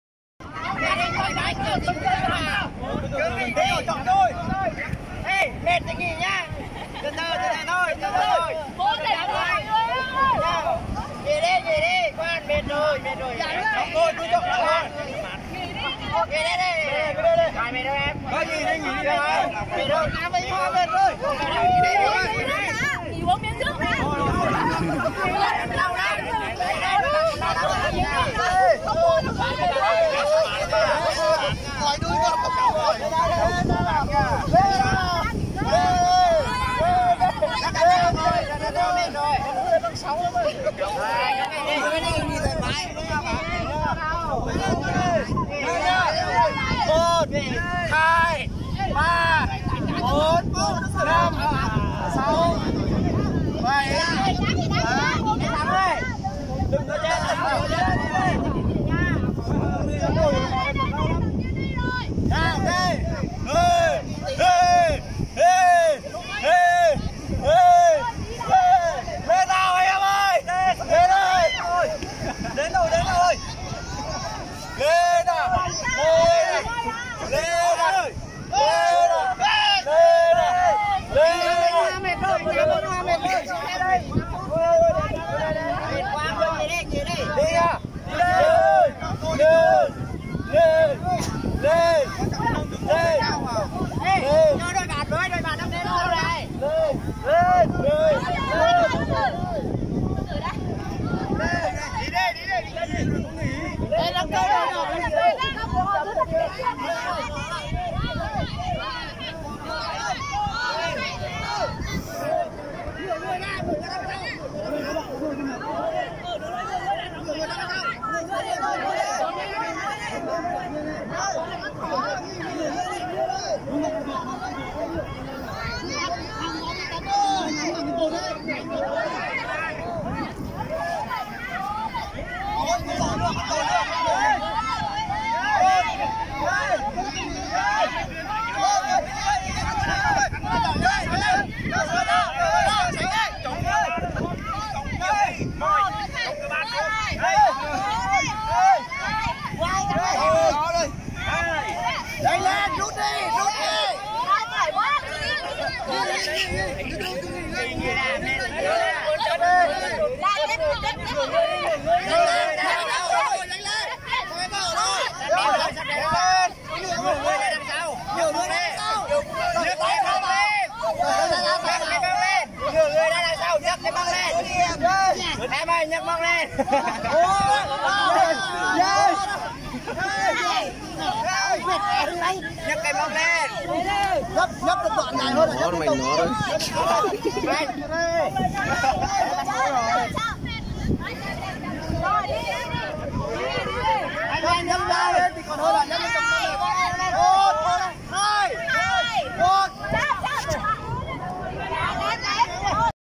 Team Building bãi biển Sầm Sơn
Trò chơi vận động. Chơi xong sau một đêm ngủ dậy phê người luôn.